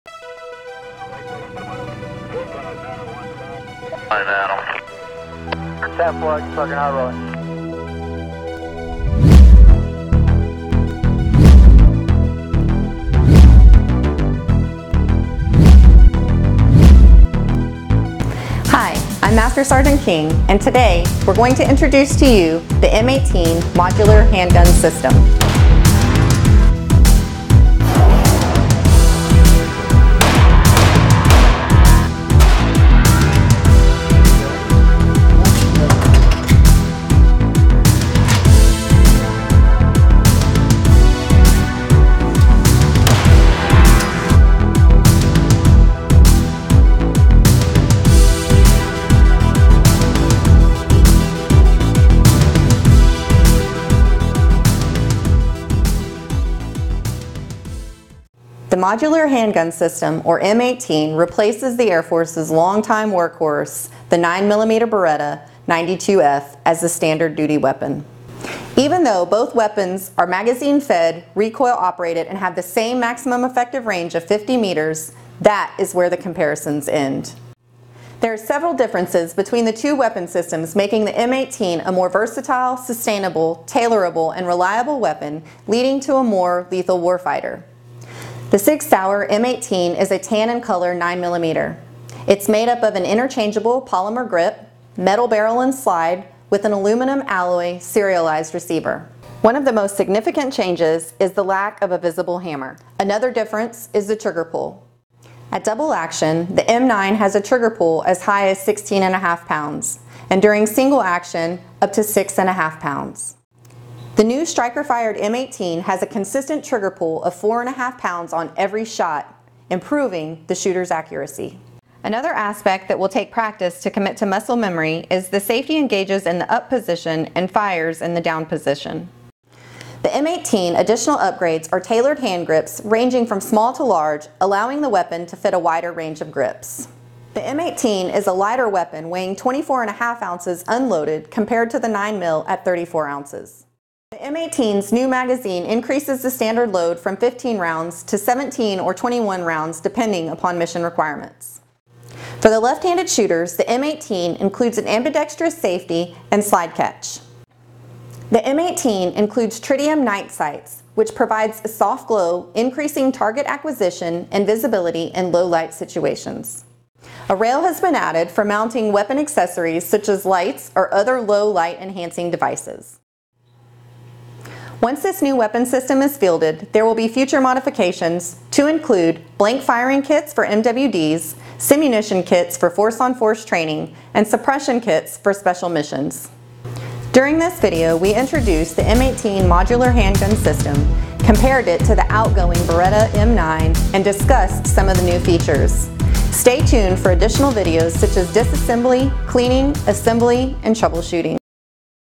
M18 Modular Handgun System
Introducing the new Air Force M18 Modular Handgun System. This video was created by the US Air Force Security Forces Center. The M18 is magazine fed, recoil operated, 4.5 lbs. every shot, and only weighs 24.5 oz. View the video to find out more.